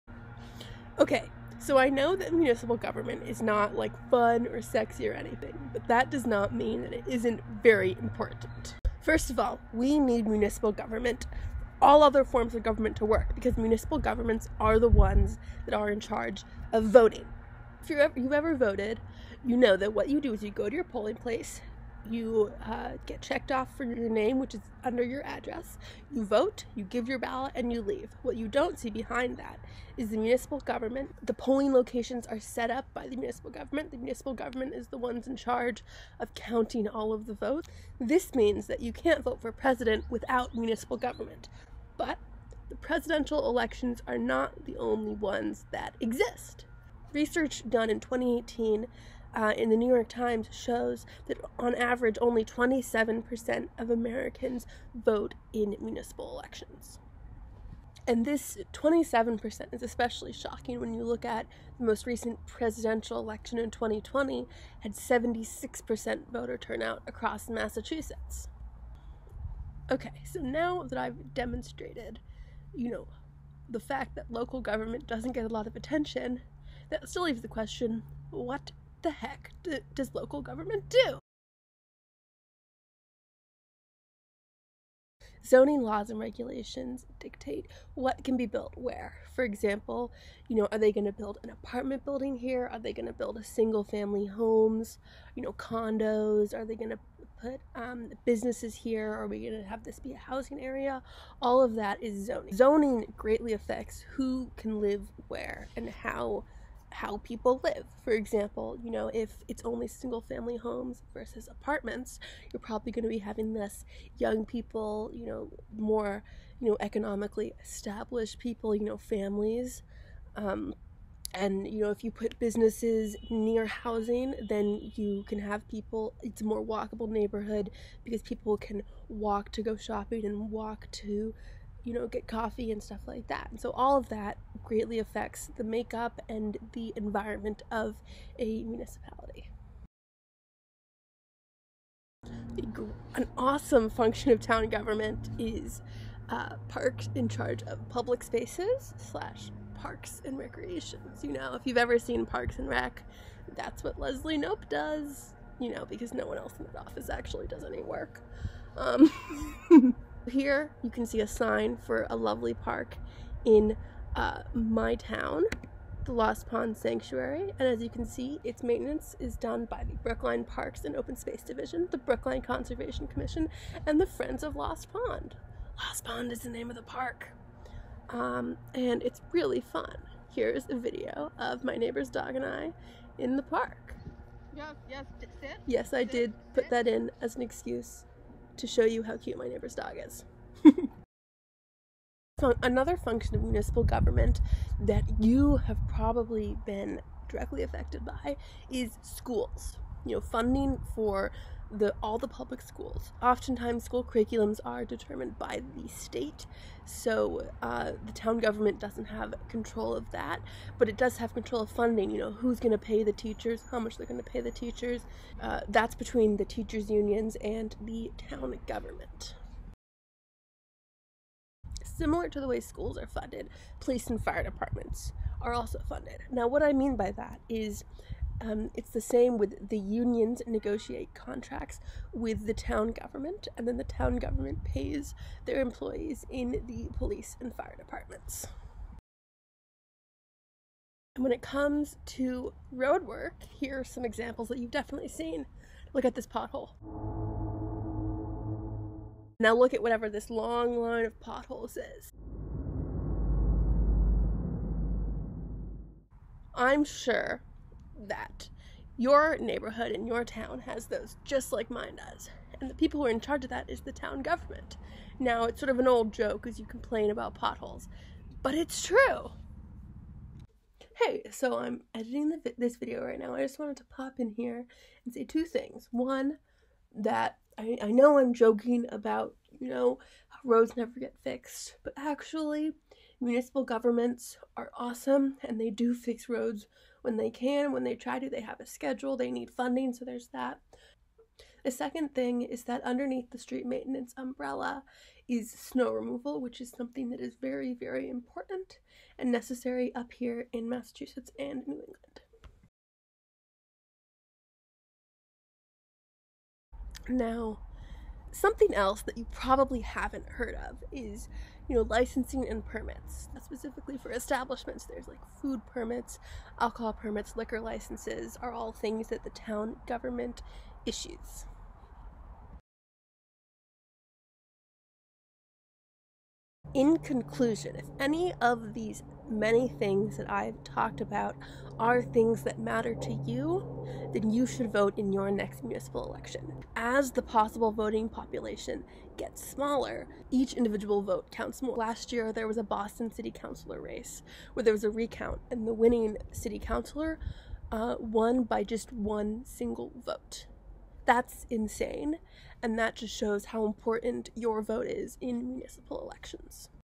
Why Local Government is important
This is my 1st vlog for COMM-262 Media Convergence